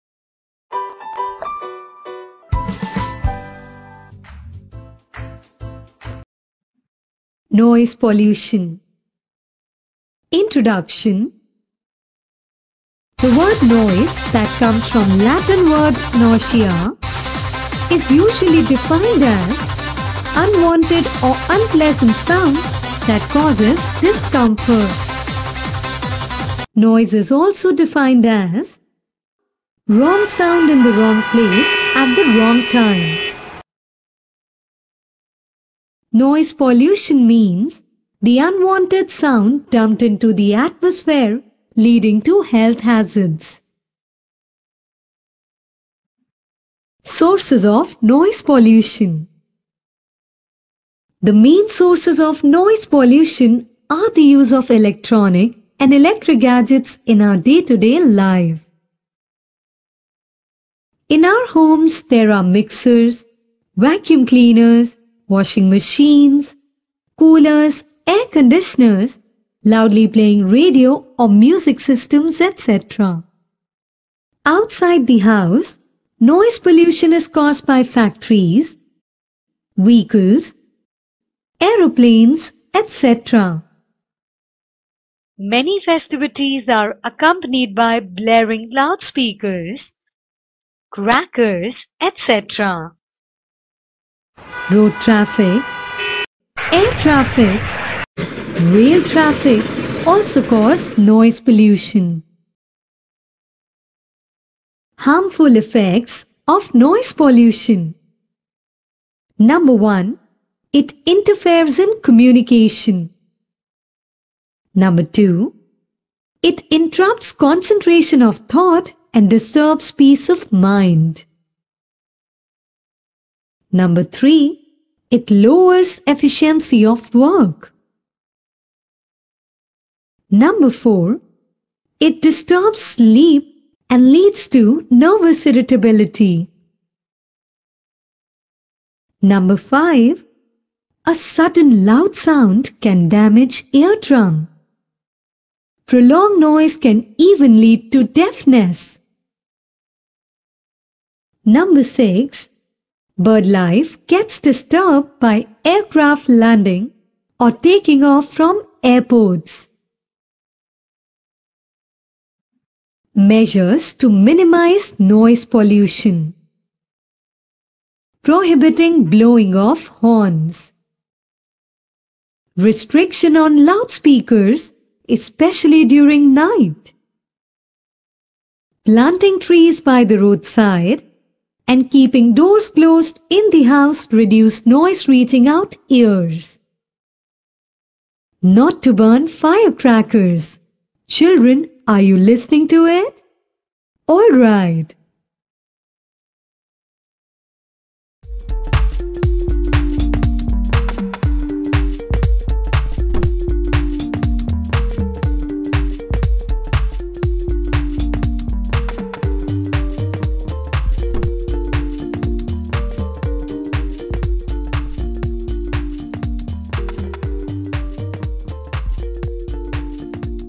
Noise Pollution | Pollution - Sources & Effects | Part 5 | Home Revise
Noise pollution refers to excessive or disturbing noise that disrupts the natural balance in the environment. Identify the major sources of noise pollution in the environment and how it affects human health. What noise control measures must we take to mitigate or reduce noise pollution?

Markers
0:00 Intro
0:46 Sources of noise pollution
1:44 Harmful effects of noise pollution
2:42 Measures to minimise noise pollution